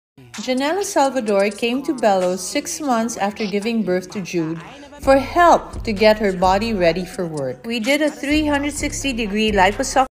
JANELLA SALVADOR SUCCESSFUL LIPOSUCTION BY VICKI BELO
Janella Maxine Desiderio Salvador, simply known as Janella Salvador, is a Filipina actress, singer and model. After appearing on stage, she debuted in the 2012 hit family series Be Careful with My Heart and since starred in Oh My G!, So Connected, Born for You and The Killer Bride.

Liposuction, or simply lipo, is a type of fat-removal procedure used in plastic surgery. Evidence does not support an effect on weight beyond a couple of months and does not appear to affect obesity-related problems. In the United States, liposuction is the most common cosmetic surgery.